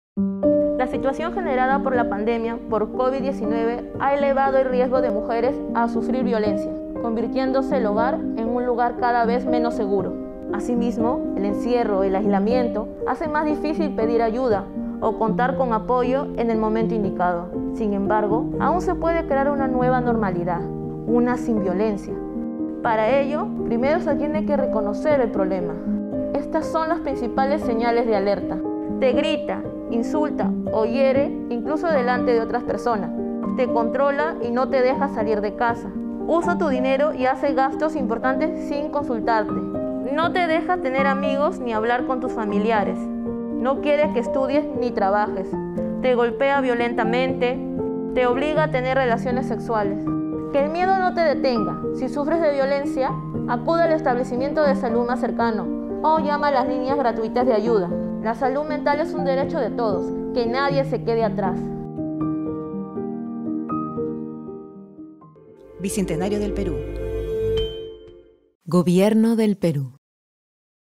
NoMásViolencia | Señales de alerta de violencia de mujer
☝🏻 Es importante reconocer las señales de alerta de violencia. Si sufres de violencia acude al establecimiento de salud más cercano o llama a las líneas gratuitas de apoyo y orientación psicológica: 113 opción 5 del Minsa o la Línea100 del Ministerio de la Mujer y Poblaciones Vulnerables del Perú📞